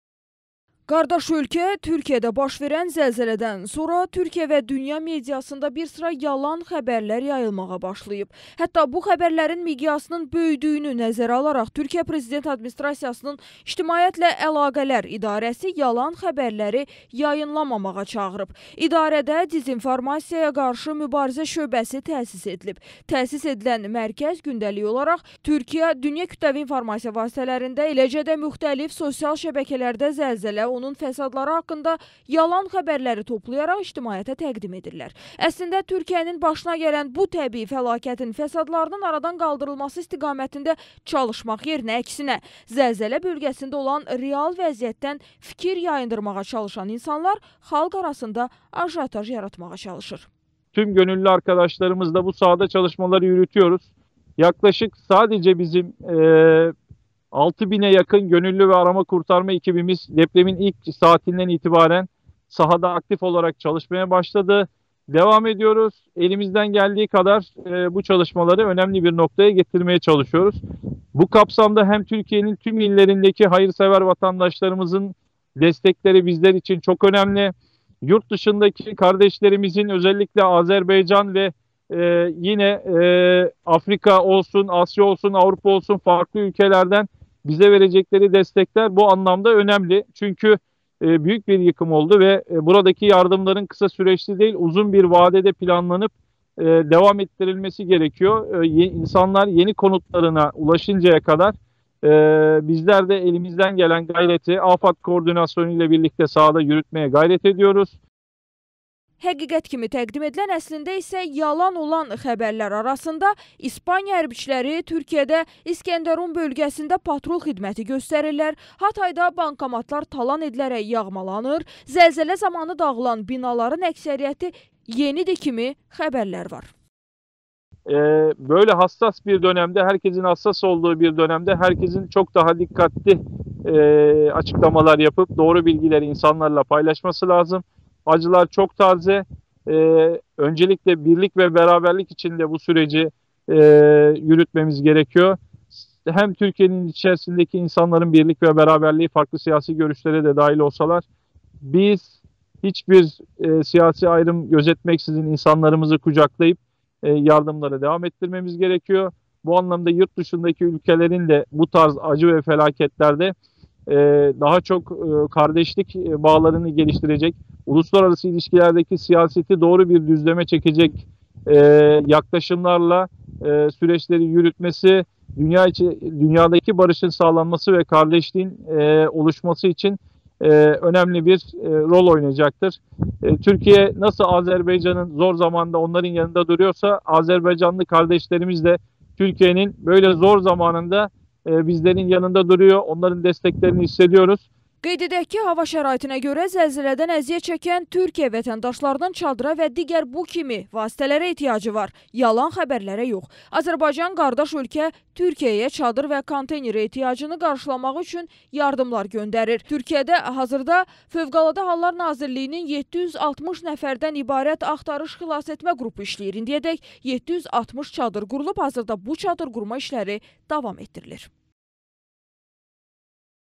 Türkiyə PA-dan xəbərdarlıq
Rəsmi Ankara zəlzələ ilə bağlı feyk xəbərlər yayımlamamağa çağırır